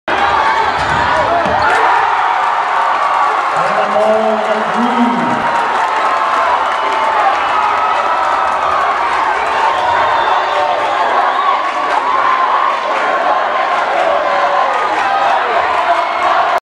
Shasta @ Enterprise 1-13-12: Caleb 3 draws sign of support
Shasta's Caleb Boyle drills a 3 ball as he and his defender both hit the floor, tying the game at 39 and prompting a Shasta cheering section to sign up!